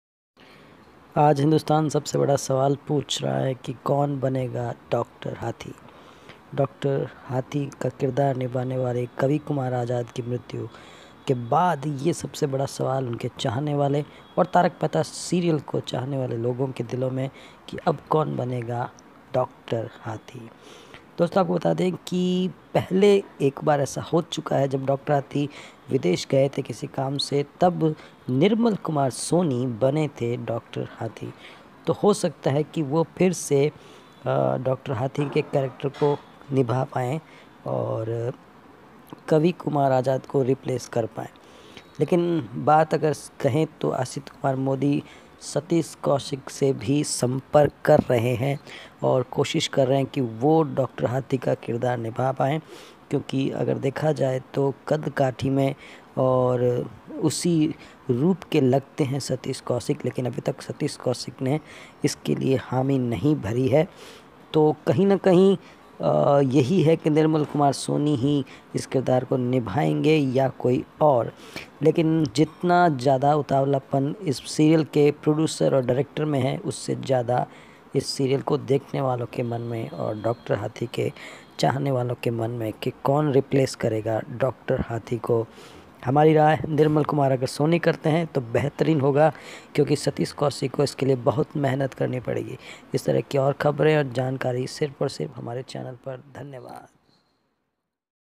कौन बनेगा डाॅ हंसराज हाथी !! सतीश कौशिक या निर्मला सोनी या कोई और
तारक मेहता का उल्टा चश्मा के डाॅ हंसराज हाथी यानि कवि कुमार के निधन के बाद एक ही सवाल हैं कि अब डाॅ हंसराज हाथी का किरदार कौन निभाएगा